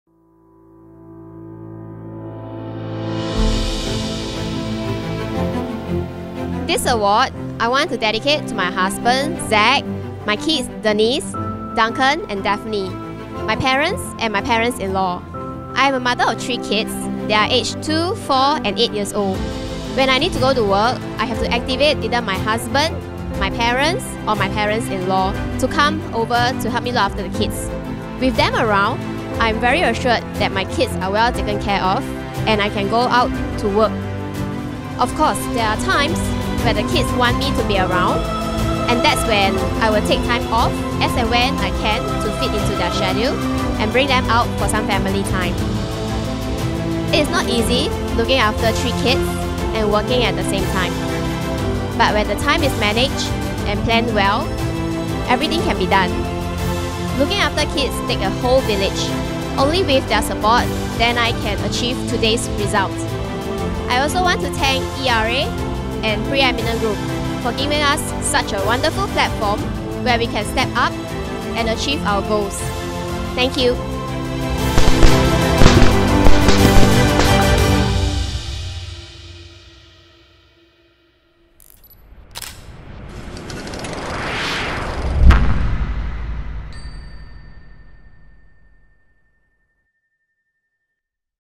Elizabeth Preeminent Gala Highlight
A high quality video done by our talented Superlime team, capturing Elizabeth's memorable time at the Preeminent Gala!